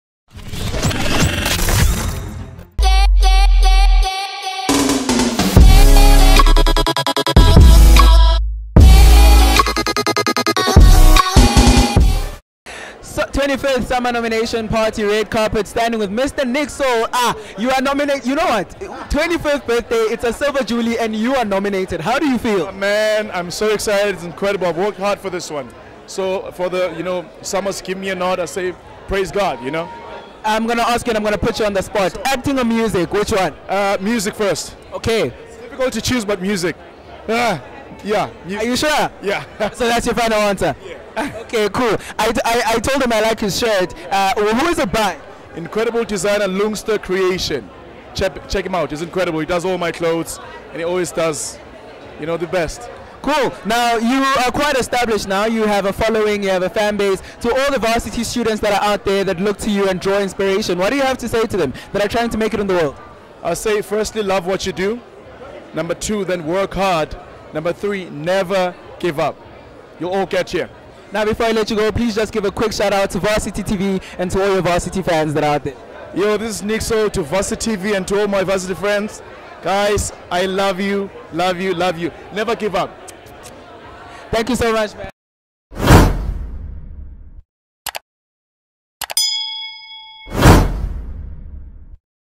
Nick Soul sends his love to all varsity fans
Rnb and Soul Singer Nick Soul sends a shout out to all his Varsity Fans and shares with them the tree things which important to make in life: Never giving up, Work hard and love what you do.